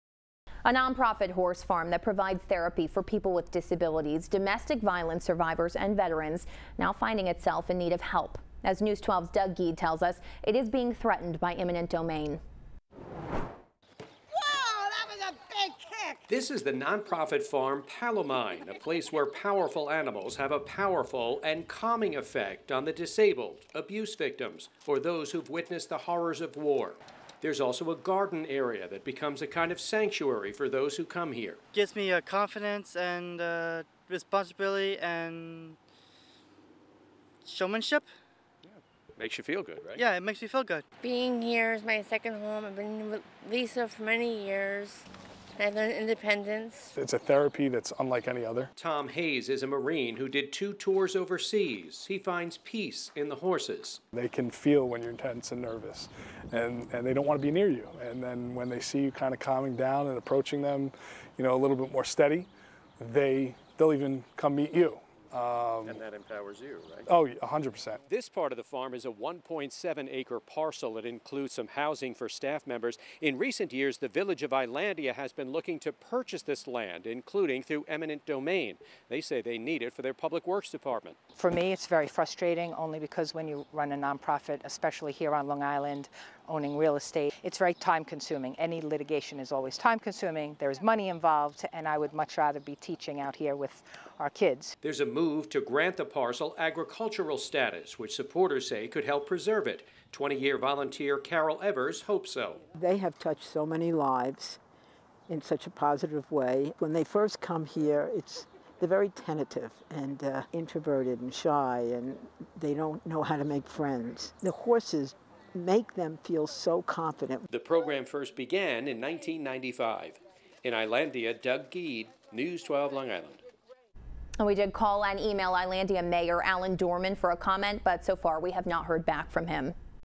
Eminent domain puts part of therapeutic horse farm in Islandia in jeopardy
Pal-O-Mine is a place where animals have a powerful and calming effect on the disabled, abuse victims or those who've witnessed the horrors of war. There's also a garden area that becomes a kind of sanctuary for visitors.